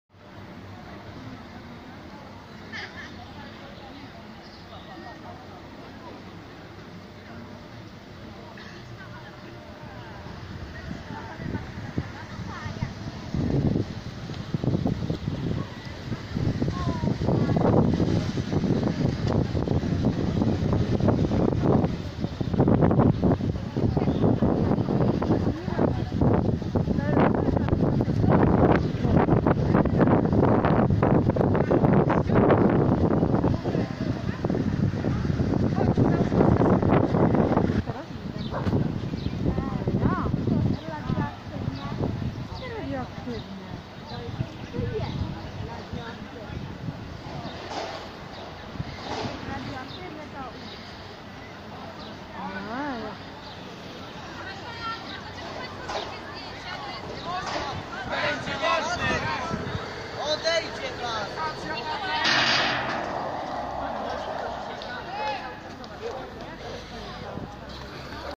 Warszawska Straż Pożarna. Barska 2-4
20.05.20r. g.16.37. działania zastępów 251w22 i 303w25, i innych służb.